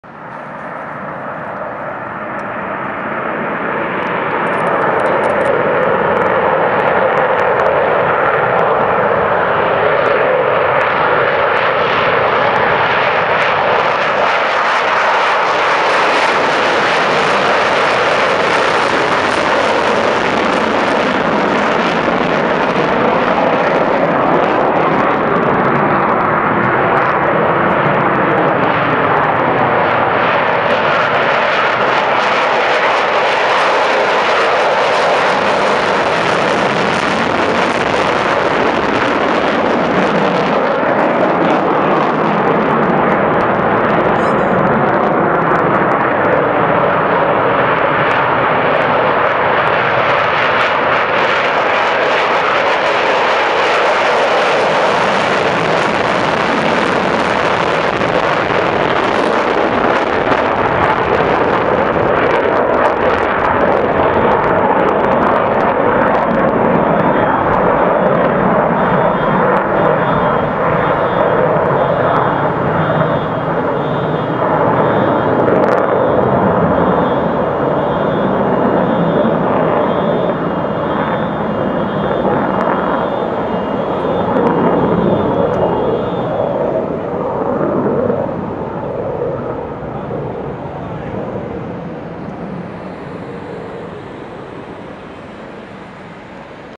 Full Afterburner!!! 3 USAF McDonnell Douglas F-15E Strike Eagles Takeoff From PDX
Probably my best military catch at PDX for a long time!